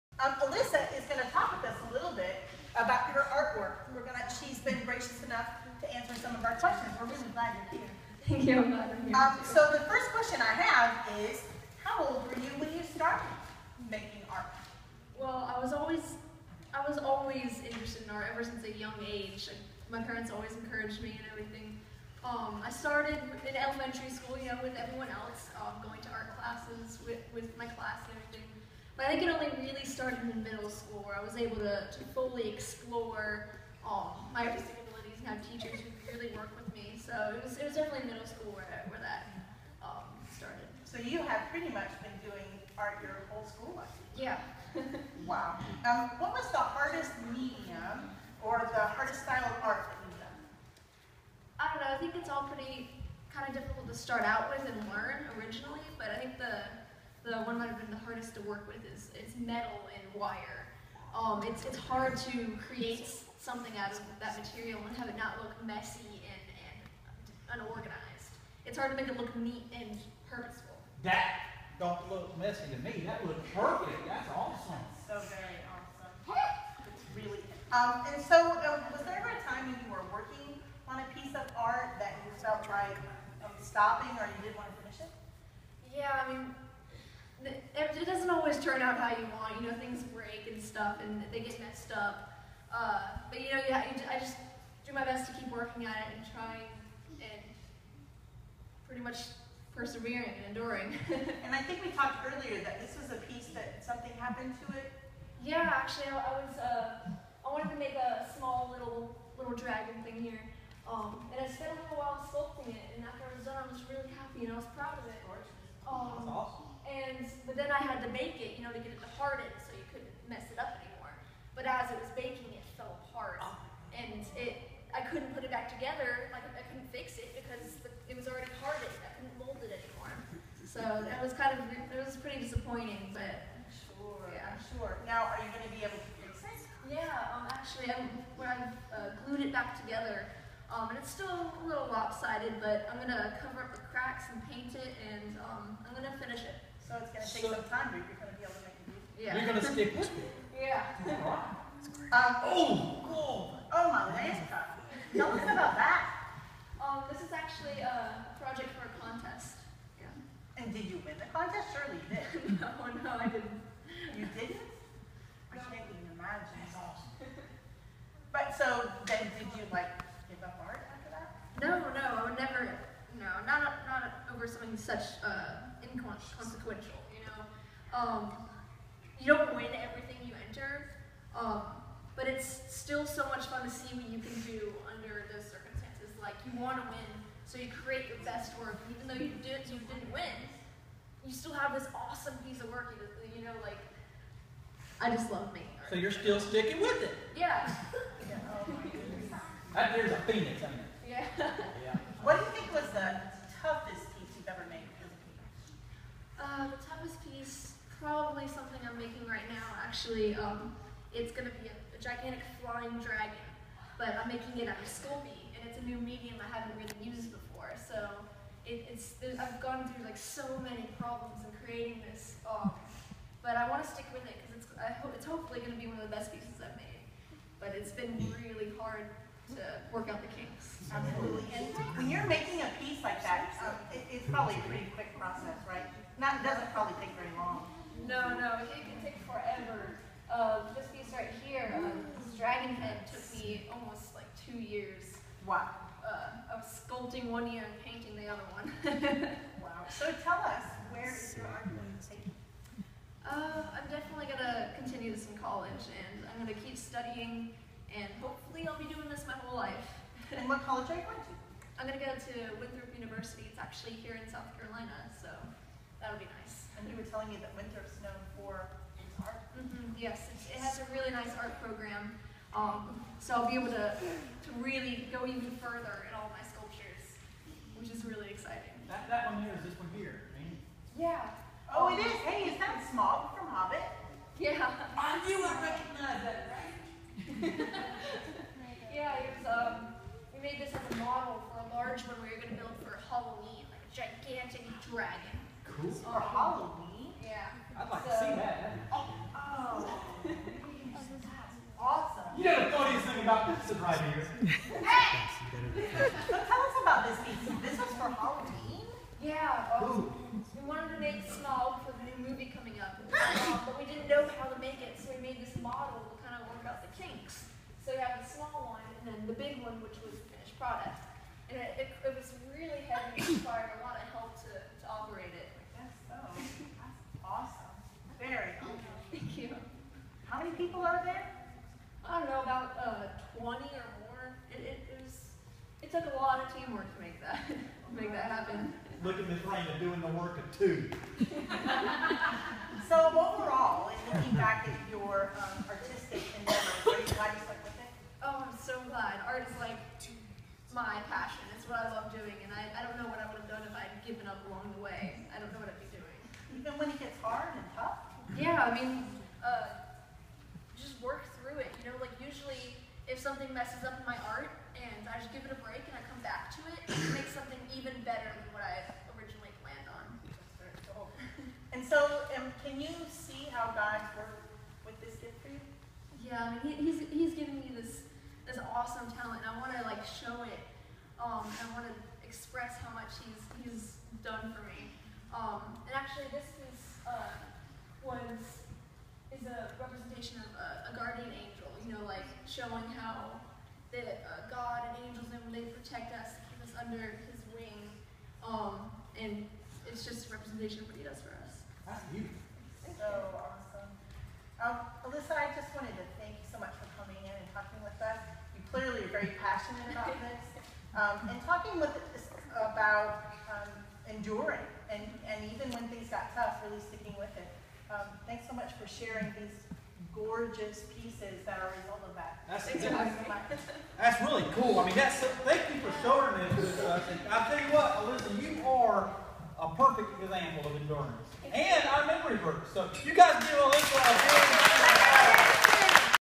Alyssa in Surge
Alyssa was interviewed in children's church about her art. They were doing a lesson on endurance from Galatians 6:9.